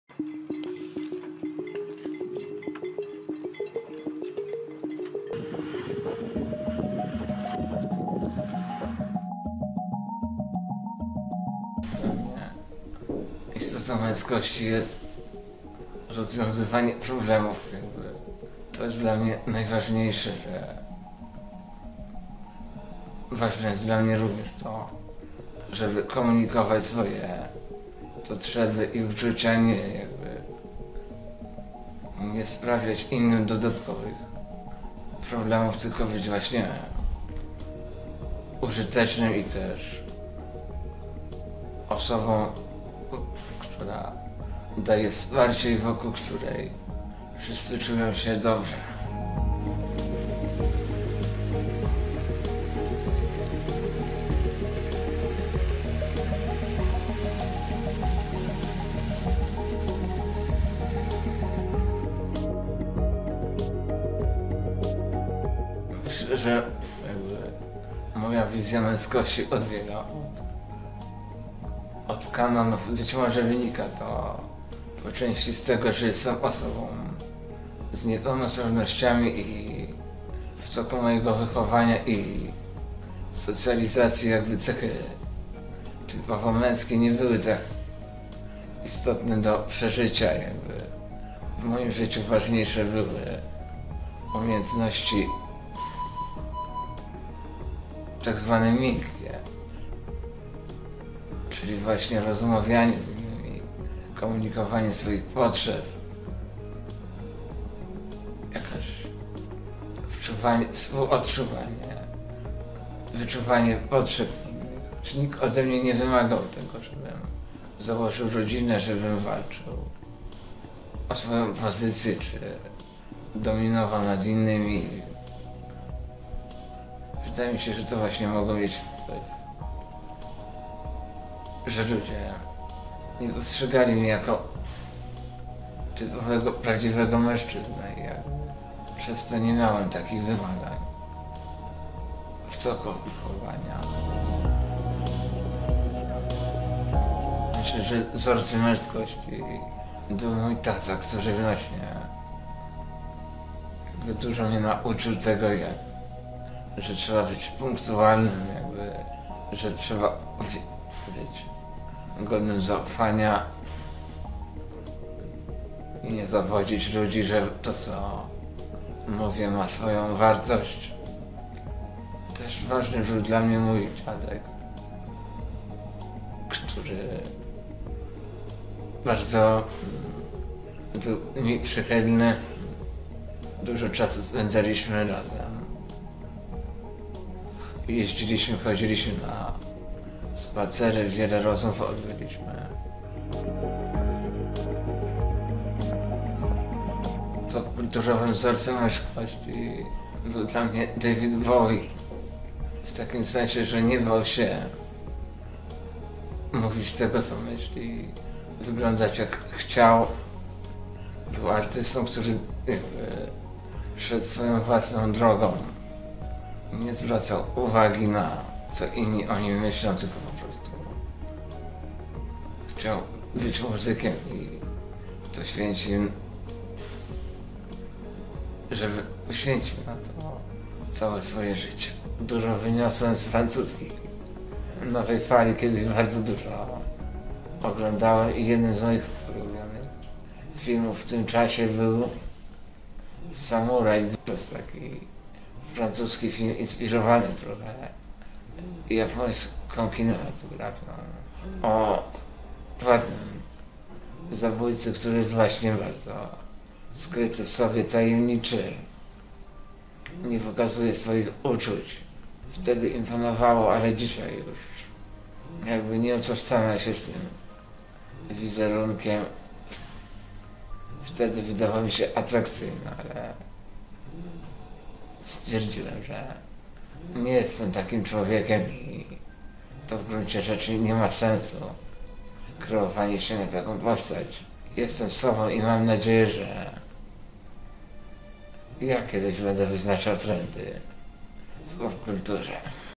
Niepełnosprawność a męskość
Jaki wpływ na postrzeganie męskości i bycie mężczyzną może mieć niepełnosprawność? Wojtek Sawicki (Life On Wheelz), influencer i aktywista z Gdyni opowiedział – o zmieniających się na przestrzeni lat – swoich wzorcach męskości.

⚫️ Więcej na temat męskości w reportażu multimedialnym, który w ramach ENTR_pl przygotował NOIZZ.pl